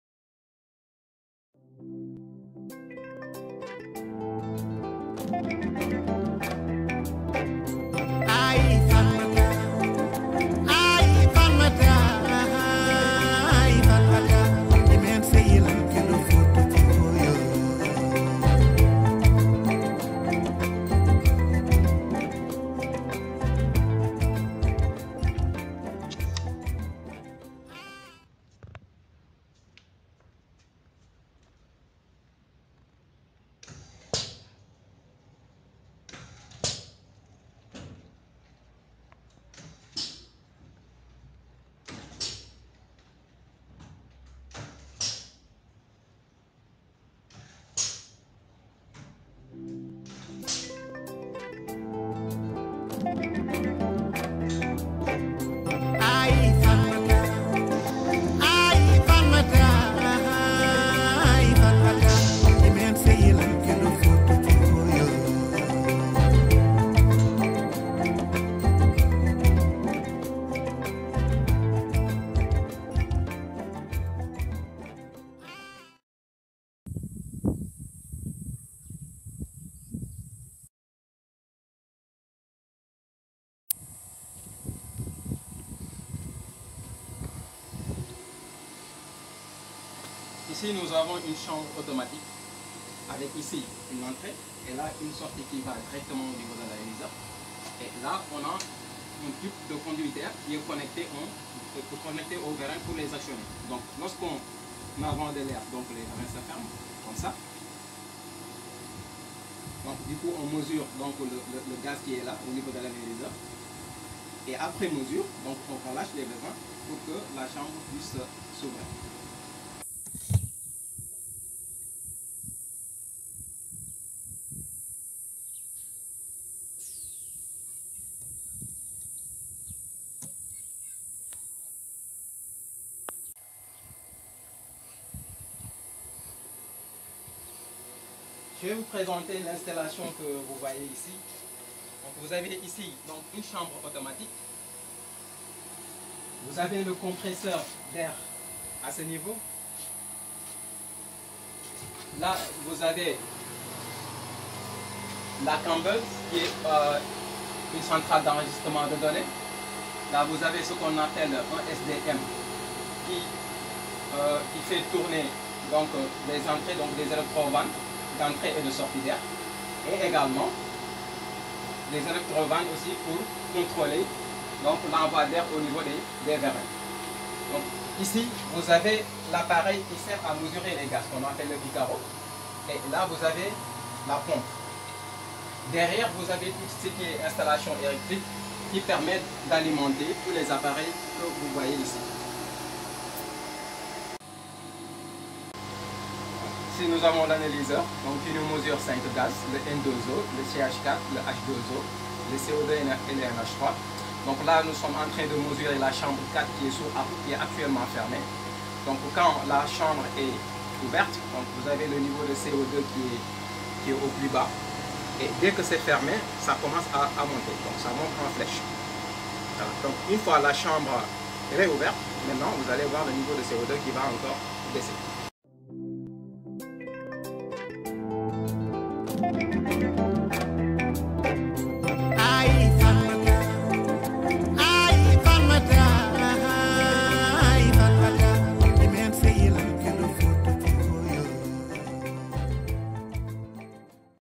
CaSSECS : Test de mesure automatique des flux du sol de 5 gaz à effet de serre (GES)
Test du dispositif de mesure automatique des flux du sol de 5 gaz à effet de serre (GES) dans le système (agro) sylvopastoral de Niakhar (bassin arachidier du Sénégal). Un dispositif constitué de chambres automatiques de mesure de GES du sol, de boite de contrôle de l'automate avec module cambel scientifique, d'analyseur piccaro, de pompe recirculante, d'électrovannes pour actionner les clapets des chambres, d'abri et panneaux solaires, entre autres.